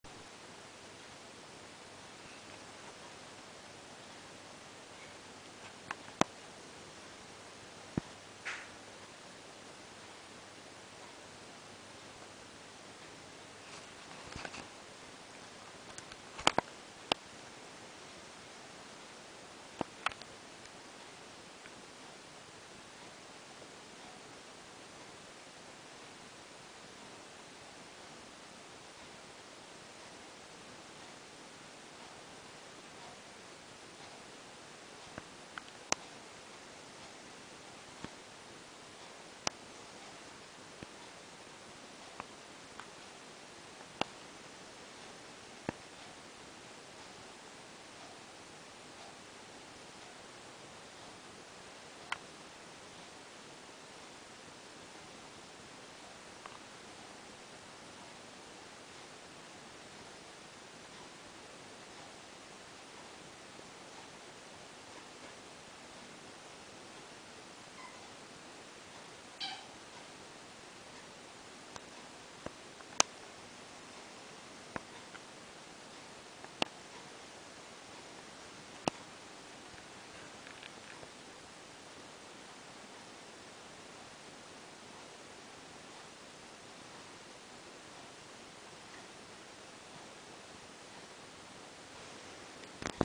Pippi checks the oven
Pippi in our kitchen. Checks the oven as there seems to be another kitty in there...